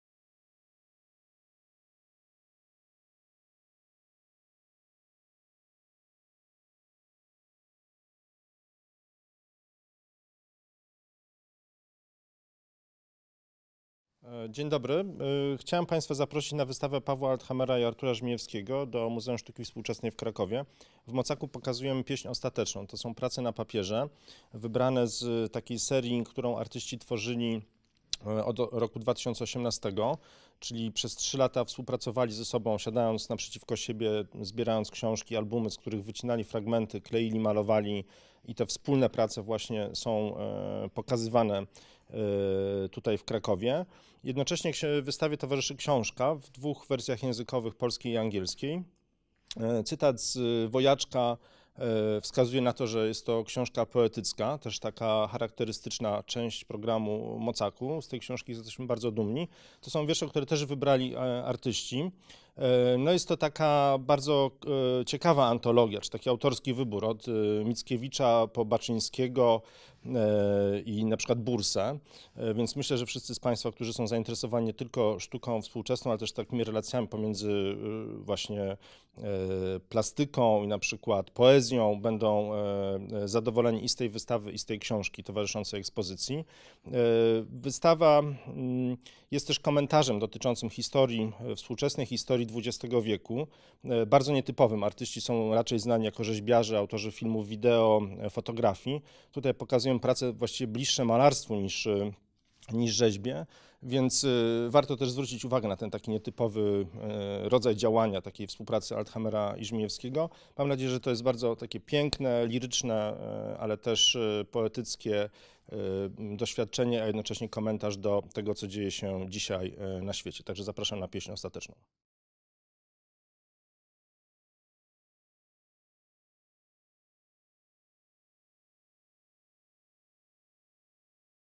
Paweł Althamer, Artur Żmijewski „Pieśń ostateczna” | 'The Ultimate Song' | Napisy PL, ENG, PJM
Wystawa Pawła Althamera, Artura Żmijewskiego „Pieśń ostateczna”
Kurator: Adam Mazur
Koordynator: Mirosława Bałazy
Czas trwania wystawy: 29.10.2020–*
Miejsce: Galeria Beta

Na wystawie „Pieśń ostateczna” Pawła Althamera i Artura Żmijewskiego prezentujemy wybór 20 kolaży powstałych w latach 2017–2019.
Artyści, absolwenci pracowni Grzegorza Kowalskiego, oddani idei Formy Otwartej Oskara Hansena, debiutowali w latach 90. Od tamtego czasu wpisują się w kanon sztuki polskiej i światowej. Od ukończenia studiów w Akademii Sztuk Pięknych w Warszawie pozostają w kontakcie, przyjaźnią się, wystawiają razem i współpracują. Podczas wspólnej rezydencji w Japonii rozpoczęli cykl prac na papierze, z wykorzystaniem tuszu i stempli, a od 2017 roku tworzą kolaże odwołujące się do dzieł poetów. Inspirują się między innymi Czesławem i Oskarem Miłoszami, Paulem Celanem, Stanisławem Grochowiakiem, Rafałem Wojaczkiem.
Kolaże zbudowane są z fragmentów wyciętych z książek – począwszy od erotycznych i pornograficznych publikacji Taschena przez albumy przyrodoznawcze i anatomiczne po atlasy historyczne i naukowe. Wielowarstwowe, malarskie prace mają wyraźną fakturę, barwę i gęstą kompozycję. Wojna, Shoah, cierpienie, erotyka to niektóre z wątków pojawiających się na wystawie. Żaden z nich nie dominuje. Zupełnie nie ma polityki, do której artyści przyzwyczaili publiczność przez lata walki na polu sztuki krytycznej. Nie ma też wezwań do zrywu czy opiewania rewolucji. Zamiast tego pojawia się refleksja o poezji i sztuce.
Artyści dokonali wyboru wierszy, które znalazły się w towarzyszącej wystawie publikacji, przygotowanej w dwóch wersjach językowych: polskiej i angielskiej.
Opis deskryptywny filmu:
Na ekranie Adam Mazur, kurator wystawy. Jest ubrany we wzorzystą, granatową koszulę oraz ciemną marynarkę w delikatną kratę. Stoi na jasnym tle. W prawym dolnym rogu ekranu tłumaczka PJM. Pojawia się seria zdjęć prezentujących przestrzeń ekspozycji. Następnie na ekranie pojawia się okładka katalogu wystawy oraz seria ujęć ukazujących zawartość publikacji.

Paweł Althamer, Artur Żmijewski 'The Ultimate Song' exhibition
Curator: Adam Mazur
Co-ordinator: Mirosława Bałazy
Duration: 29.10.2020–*
Venue: Beta Gallery

At the exhibition The Ultimate Song by Paweł Althamer and Artur Żmijewski we present a selection of 20 collages created between 2017 and 2019.
The artists, both graduates of Grzegorz Kowalski’s studio and dedicated to Oskar Hansen’s concept of the Open Form, debuted in the 1990s. They have been part of the canon of Polish and world art ever since. They have kept in touch with each other after graduation from the Academy of Fine Arts in Warsaw, and they remain friends, working together and presenting joint exhibitions. During their shared residence in Japan, they started a series of works on paper, using ink and stamps, and in 2017 they began to create collages, referencing works of poetry. The poets they find inspirational include Czesław and Oskar Miłosz, Paul Celan, Stanisław Grochowiak, and Rafał Wojaczek. The collages are made of excerpts cut out from books – starting with Taschen’s erotic and pornographic publications, through nature and anatomical albums, to historical and scientific atlases. The multi-layered, painterly works have a clear texture, colour and dense composition. War, the Shoah, suffering and erotica are some of the themes that appear in the exhibition, with no particular emphasis on any single one. There is ab absence of politics – the topic that the audiences have grown accustomed to following the artists in their years of strife in the field of criticism.  Nor are there exhortations to rise up or eulogies of the revolution. Instead, there is a reflection on poetry and art.
The artists selected the poems to be included in the accompanying publication, which is available in two versions: in Polish and in English.

* W związku z czasowym zamknięciem muzeów z powodu pandemii COVID-19 czas trwania wystawy został przedłużony. Data zamknięcia ekspozycji będzie opublikowana na stronie internetowej Muzeum

* Due to the COVID-19 pandemic, the closing date of the exhibition has changed. for the up-to-date information.
Muzeum Sztuki Współczesnej w Krakowie MOCAK
MOCAK the Museum of Contemporary Art in Krakow